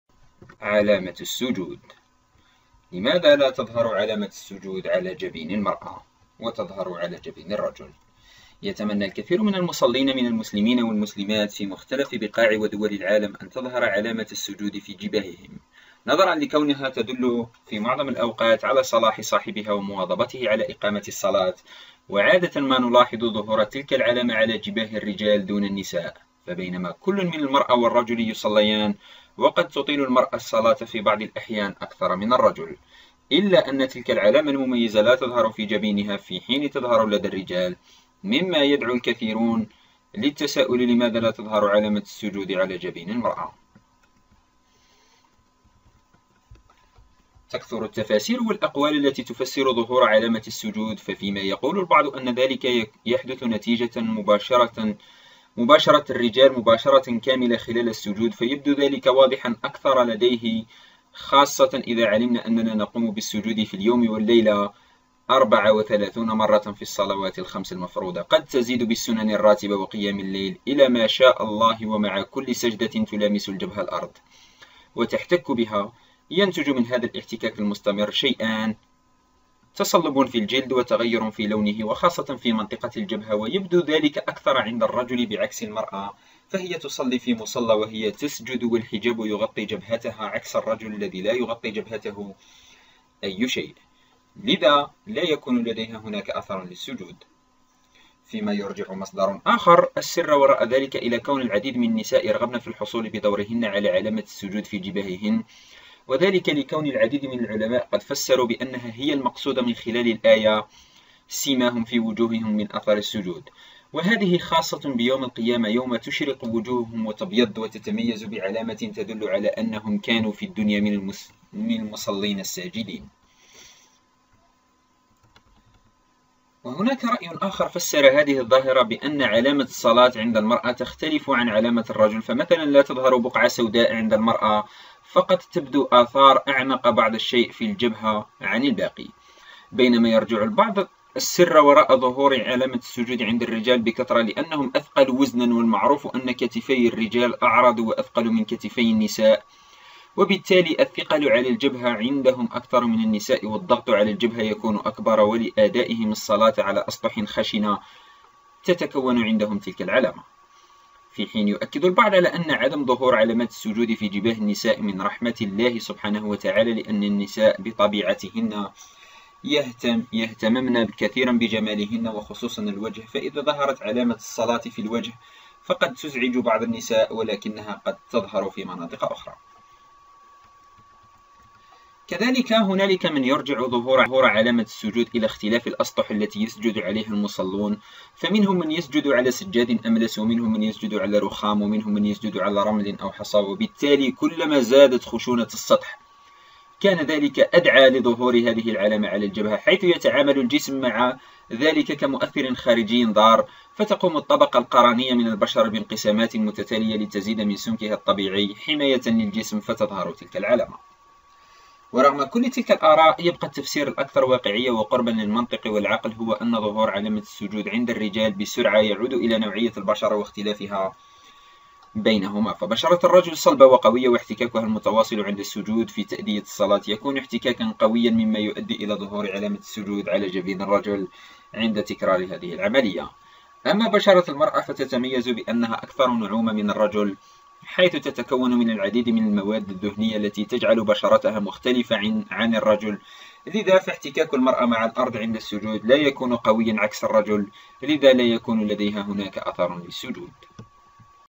لماذا لا تظهر علامة السجود على جبين المرأة ؟
يتمنى الكثير من المصلين من المسلمين والمسلمات في مختلف بقاع ودول العالم أن تظهر علامة السجود في جباههم؛ نظرا لكونها تدل في معظم الأوقات على صلاح صاحبها ومواظبته على إقامة الصلاة، وعادة ما نلاحظ ظهور تلك العلامة على جباه الرجال دون النساء فبينما كل من المرأة والرجل يصليان وقد تطيل المرأة الصلاة في بعض الأحيان أكثر من الرجل، إلا أن تلك العلامة المميزة لا تظهر في جبينها في حين تظهر لدى الرجال، مما يدعو الكثيرين للتساؤل لماذا لا تظهر علامة السجود على جبين المرأة؟